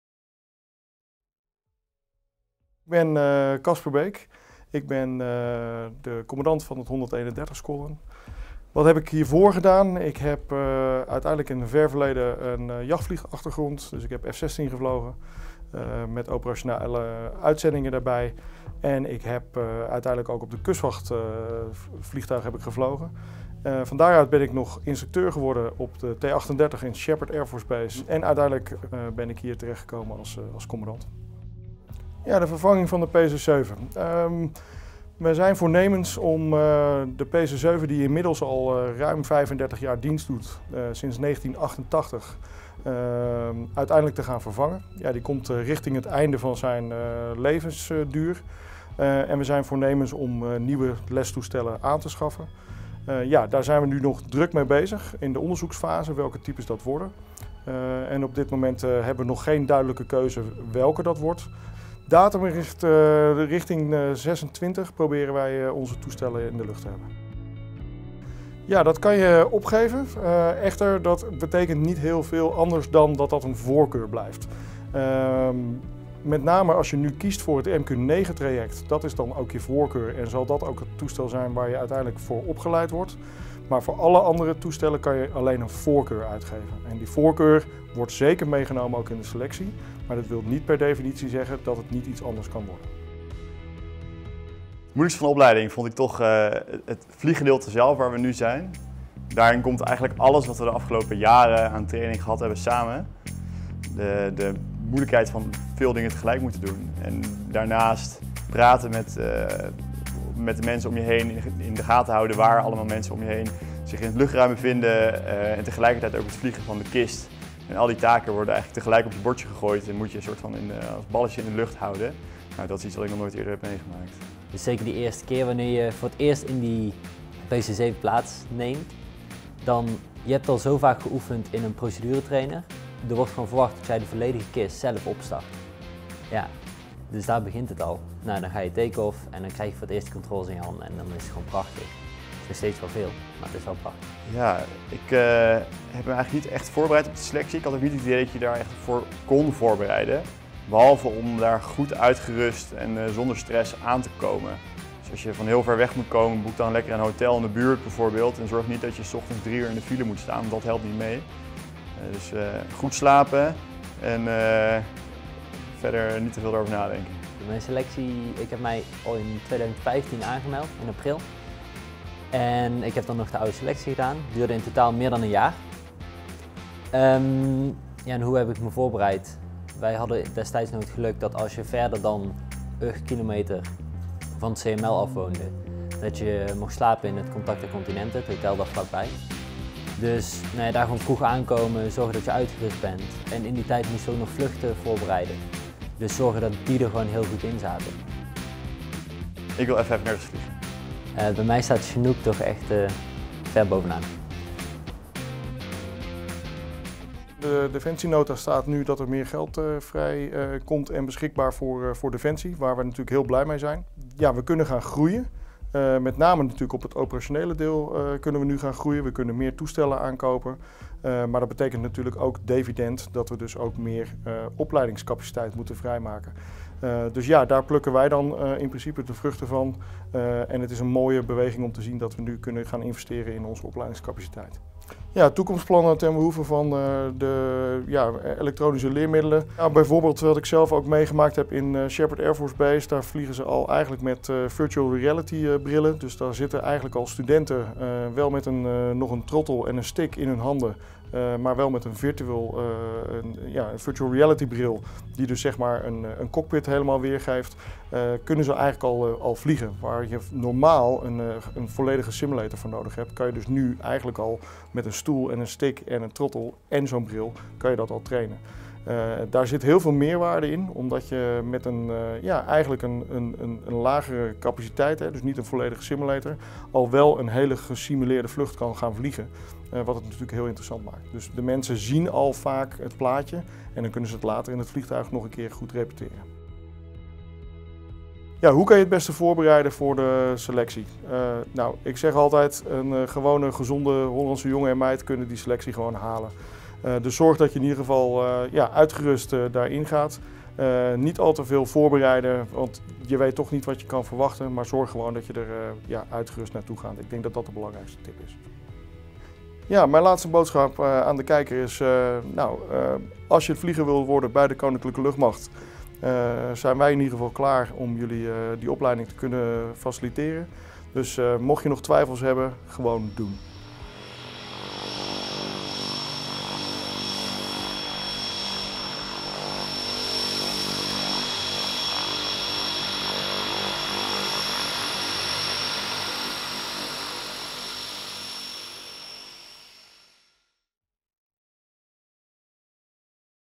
131 Squadron - Casper "Buff" over de vliegersopleiding (EMVO).
In deze video bespreekt Casper Beek (131 squadron commandant) de vragen van onze volgers op de social media.
Daarnaast komen ook de leerling vliegers aan het woord en bespreken zij wat zij hebben meegemaakt met de vliegersopleiding en waar ze uiteindelijk willen eindigen.